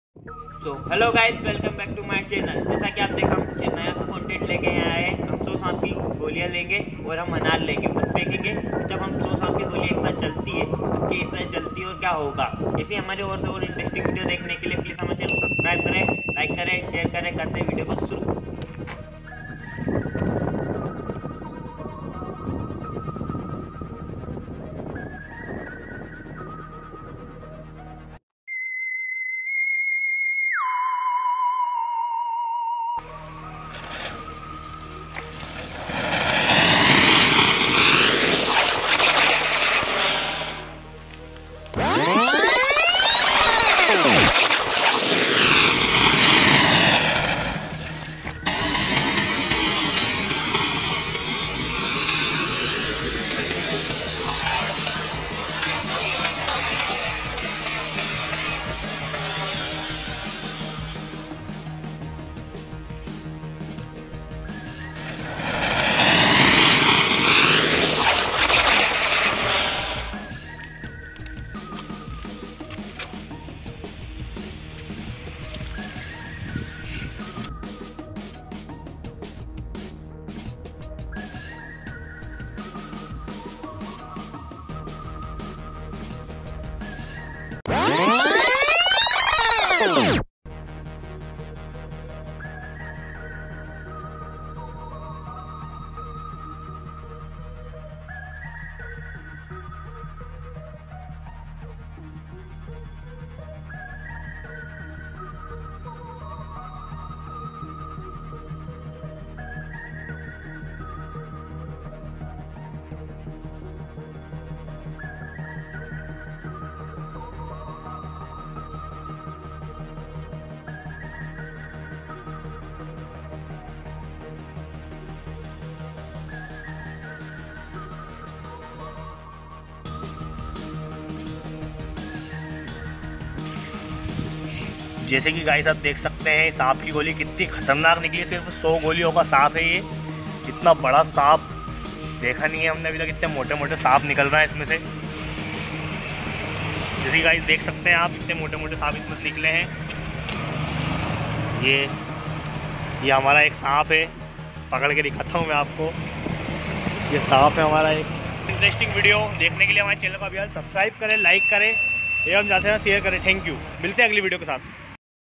Big Snake vs Fire crackers | like king cobra snake | White and black | python
Hello freinds

In this video we make a snake bomb  for only entertainment purpose
This snake bomb is making by some firecrackers Sutli bomb, Rockets, chakri, Anar, smoke bomb, And many Diwali new firecrackers...